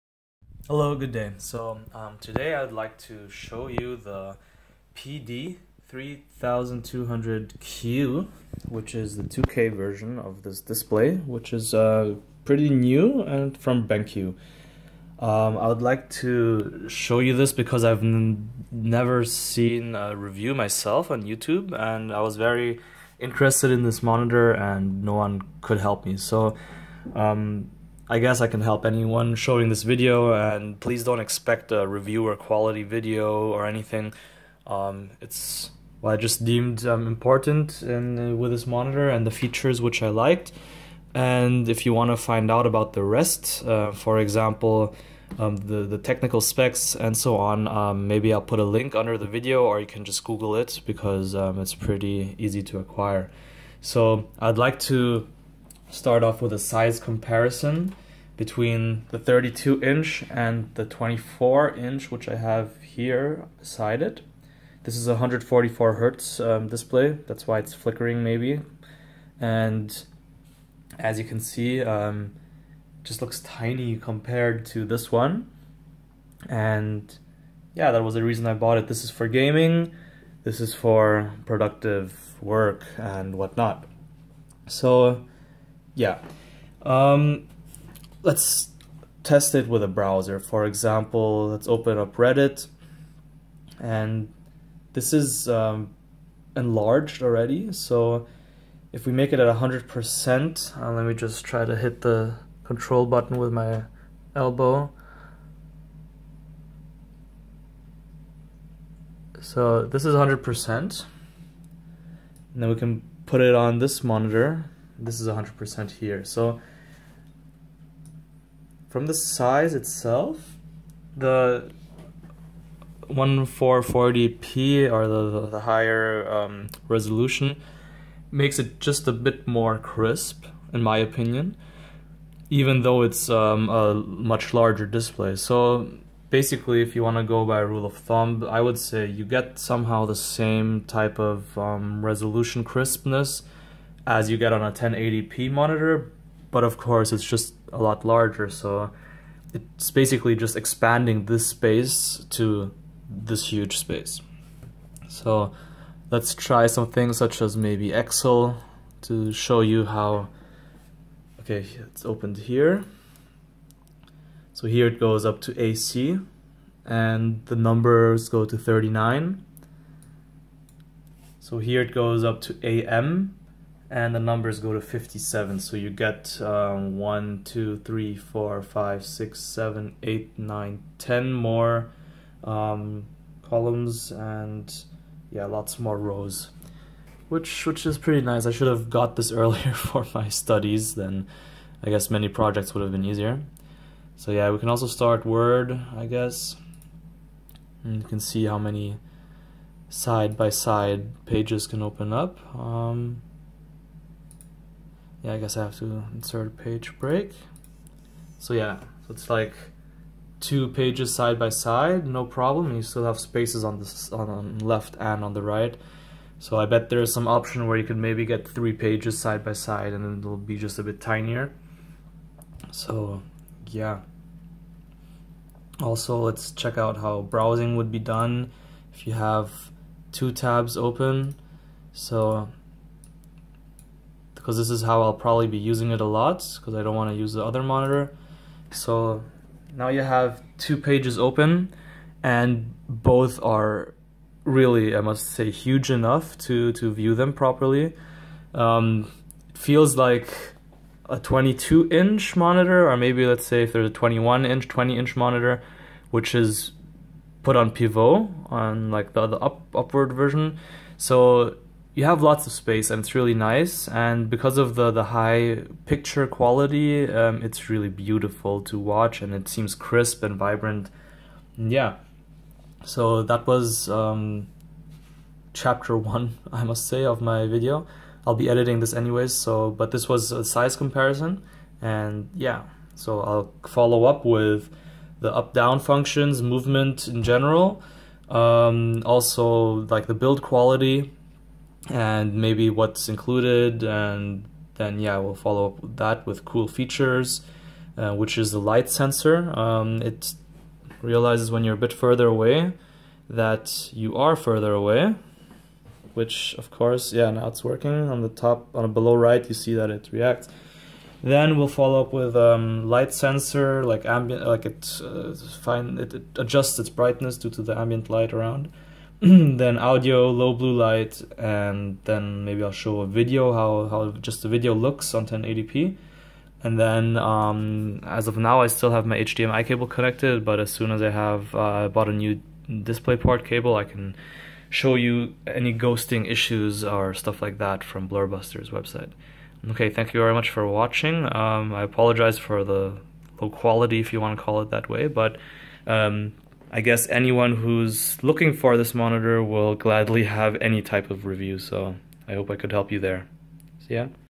BENQ PD3200Q In-Depth User Review Part 1
This is part 1,showcasing the screen size of 32 inches.
Comparison to 24 inch in excel and browsing. Word is opened up and some general info on resolution.

The other parts will include movement, build quality, cool features, video demonstration and maybe ghosting test.